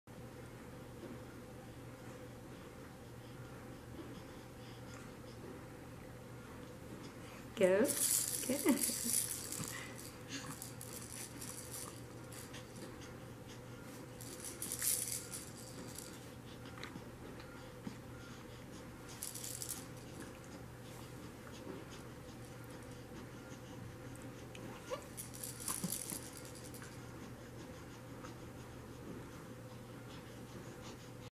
baby brain video
Tethering an infants arm facilitates cognitive and movement development.